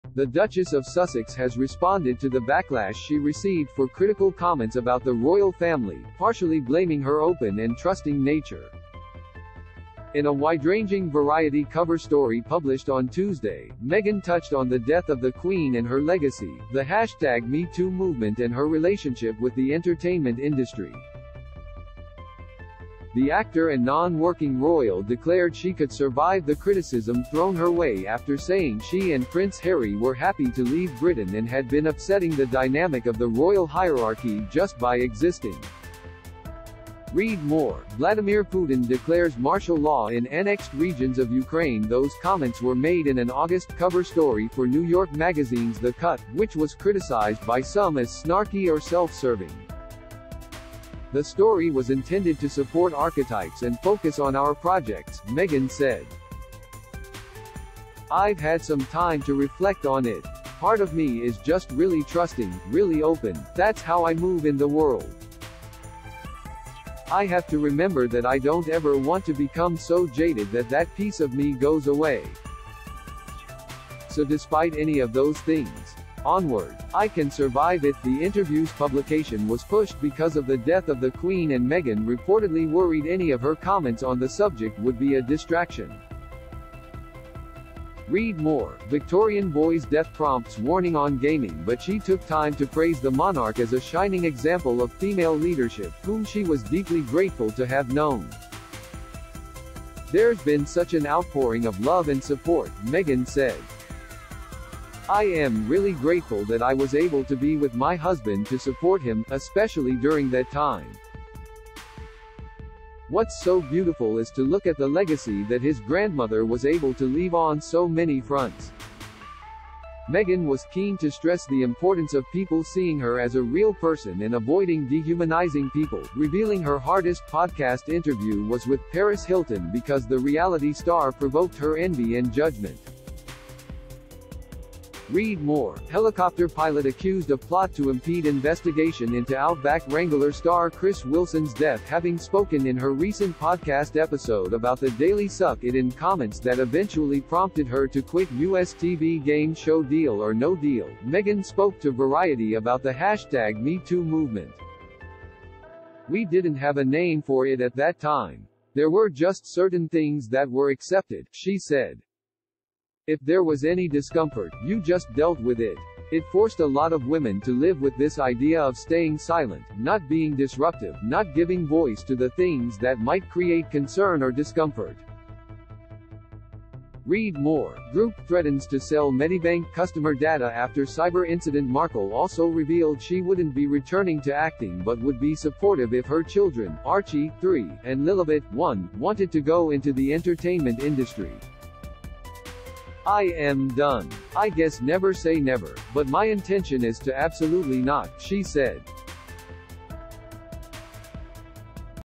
Duchess of Sussex responds to criticism over controversial interview
The Duchess of Sussex has responded to the backlash she received for critical comments about the royal family, partially blaming her "open" and "trusting" nature.In a wide-ranging Variety cover story published on Tuesday, Meghan touched on the death of the Queen and her legacy, the MeToo movement and her relationship with the entertainment industry.The actor and non-working royal declared she could survive the criticism thrown her way after saying she and Prince Harry were "happy" to leave Brit